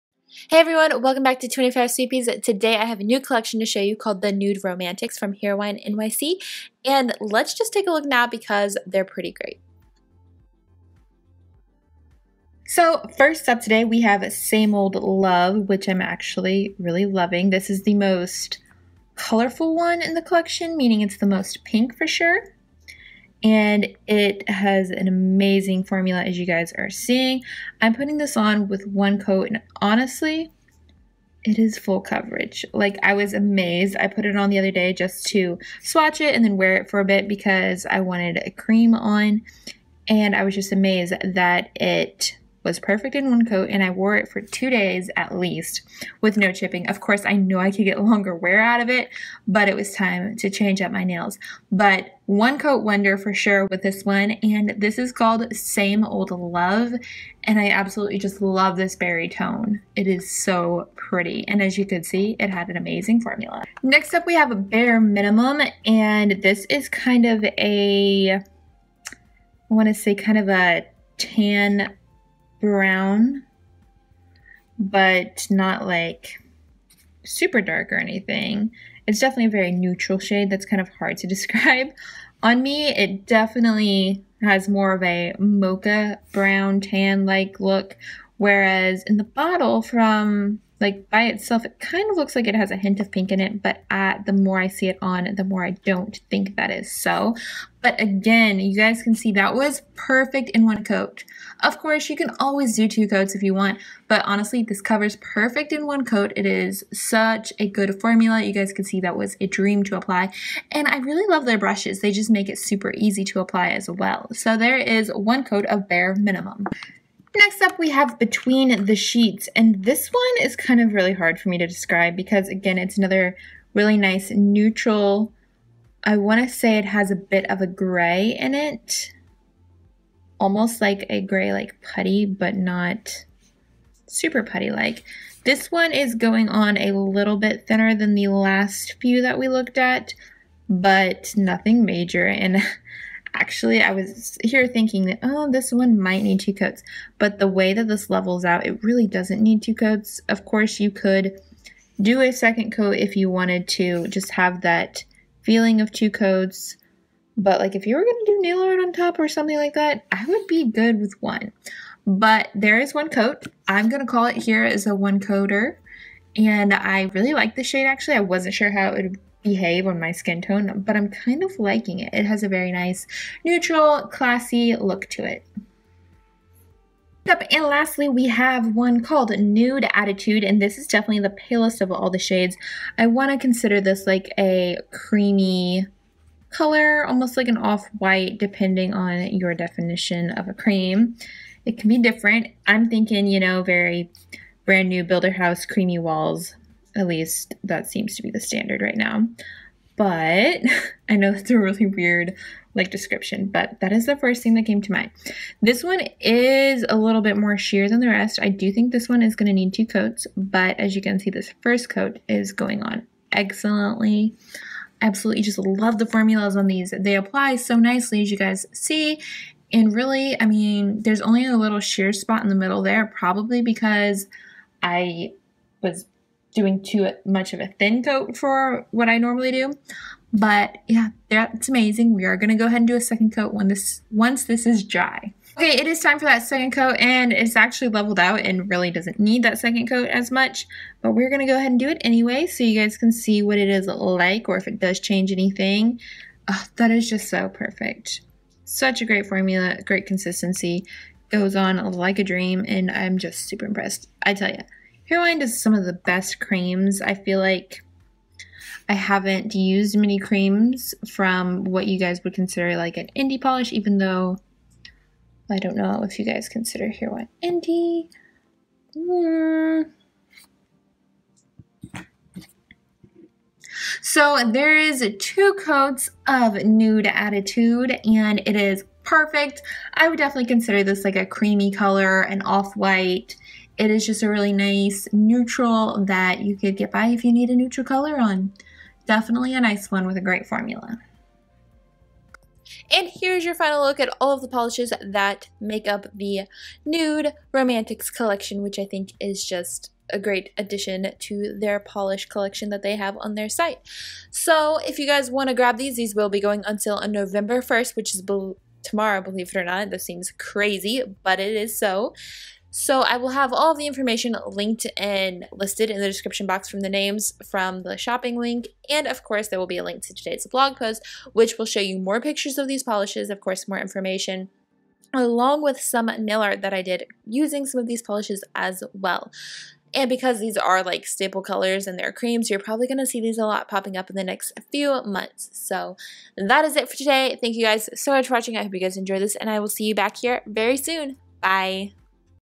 Heroine NYC The Nude Romantics | 25 Sweetpeas
New Polish Alert (Watch in HD)

The Nude Romantics Collection from Heroine NYC is coming out November 1st and consists of 4 new perfect neutral creme polishes!

Featured in this

Nude Attitude

Same Old Love

Between the Sheets

Bare Minimum

FTC: items were sent to me for review purposes